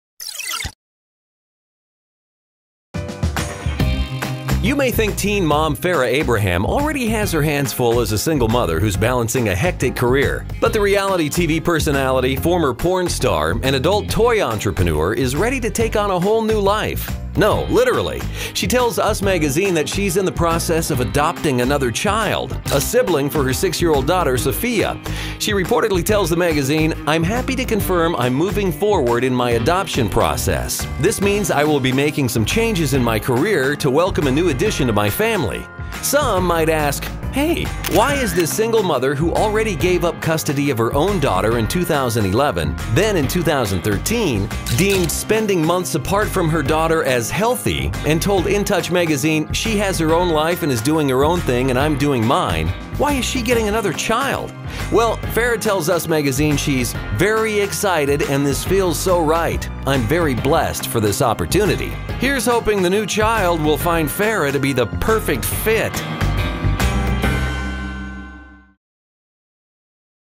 Farrah Abraham in Process of Adopting a New Child | Splash News TV
Teen Mom star and former porn actress Farrah Abraham is reportedly in the process of adopting a new child.